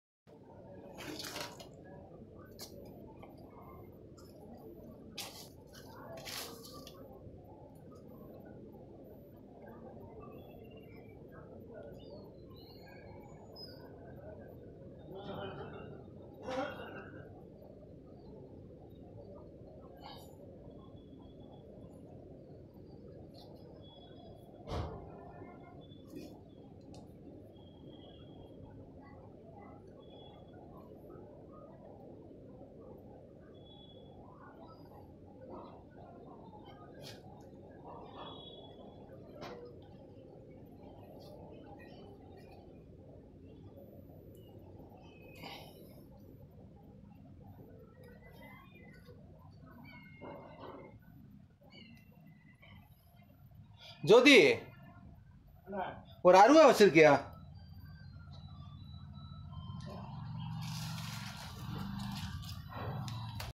variety exercises using Indian club, gadha, Mugdar ,karlakattai -for upper body strength.
This particular swing works the side delt oides primarily but also works the three deltoides in addition to this.
Anyone can try practising this swing using both hands in the beginning and then progress to swinging this with just one hand.